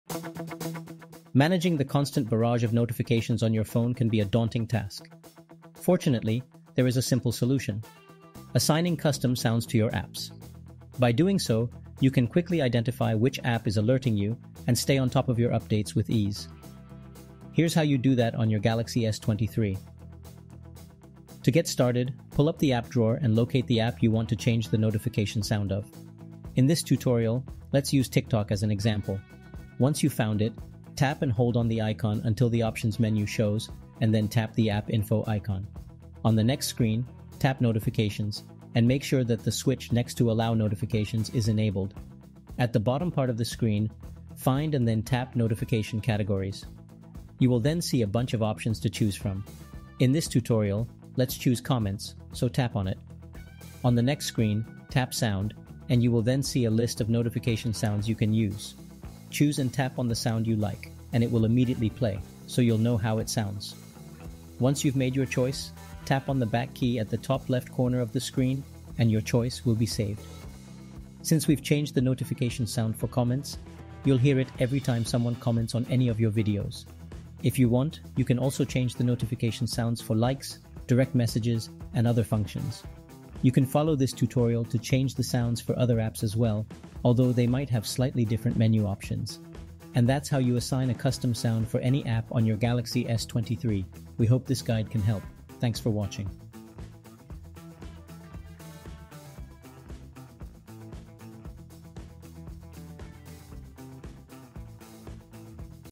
Assign Custom Notification Sounds for Specific Apps on Galaxy S23
This video tutorial aims to provide a solution to managing the overwhelming number of notifications on your phone.
By assigning custom sounds to your apps, you can quickly identify which app is alerting you and stay on top of your updates with ease.
 The tutorial walks through the steps to enable notifications for an app, select notification categories, choose notification sounds, and save the changes.
Although it uses the Galaxy S23 as an example, the general guidelines provided can be applied to other devices as well.